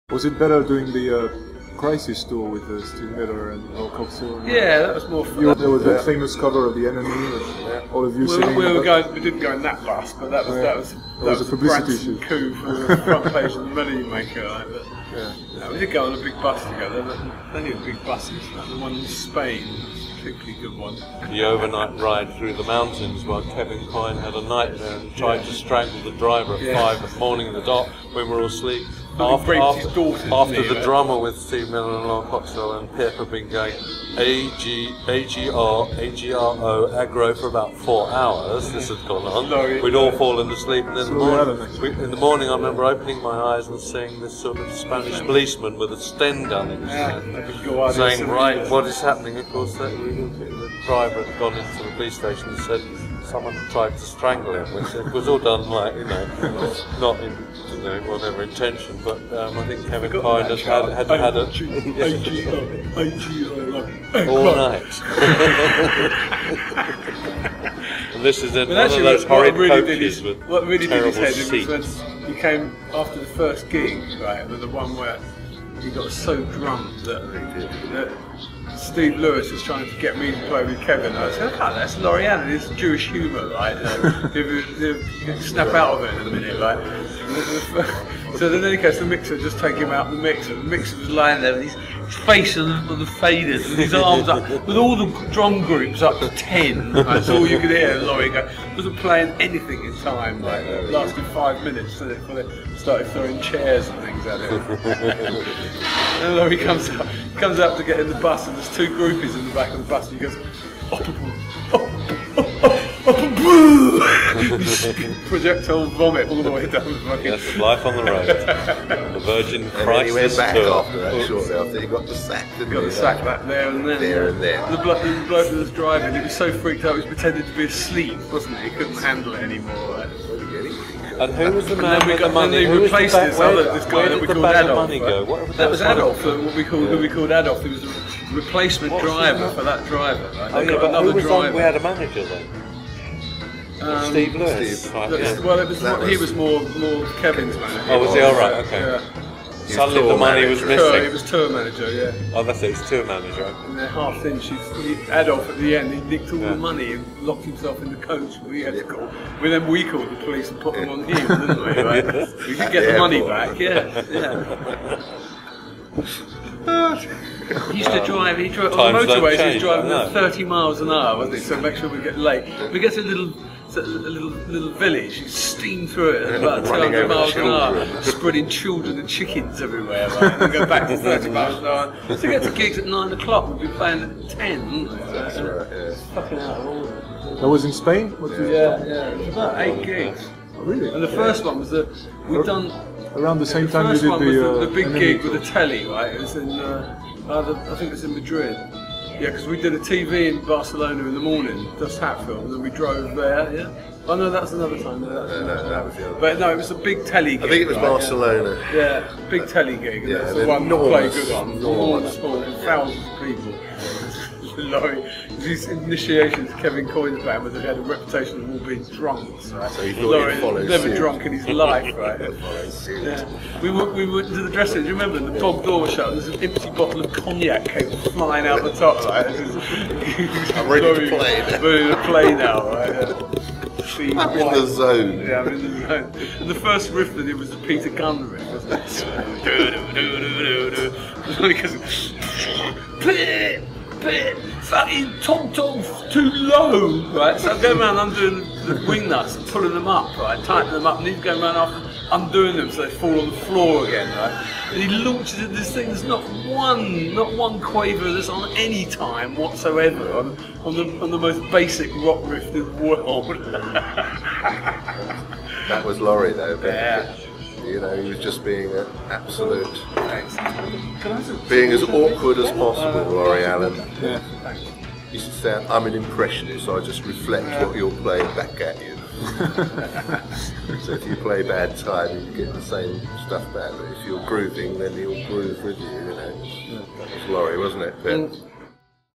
Hatfield and the North on touring Spain in 1975
In a rare trio interview, Pip Pyle, Richard Sinclair and Phil Miller remember their tour of Spain in January 1975, an extension of the Virgin Crisis Tour of the UK the previous year, with a particular focus on Virgin label mate Kevin Coyne, his drummer Laurie Allan and the filming of a TV show in Barcelona.